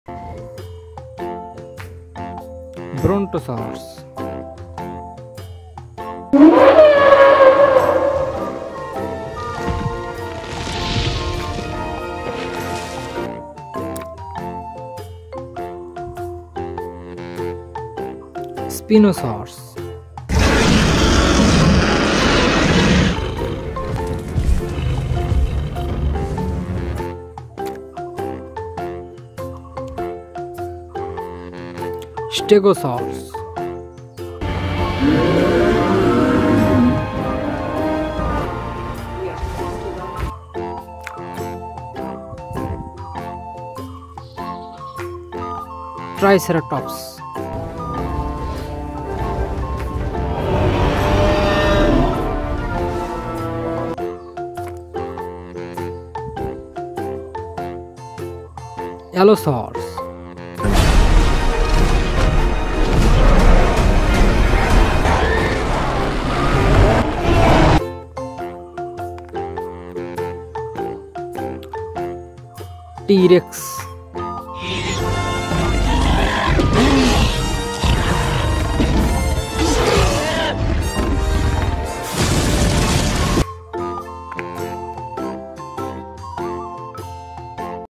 Learning Names of Dinosaurs | Brontosaurus | Stegosaurus | Spinosaurus | Triceratops | T rex
Hi friends thanks for watching if you like this video please subscribe this channel. by watching this video you will learn name of this dinosaurs, Brontosaurus, Spinosaurs, Stegosaurus, Triceratops, Allosaurus, T-Rex.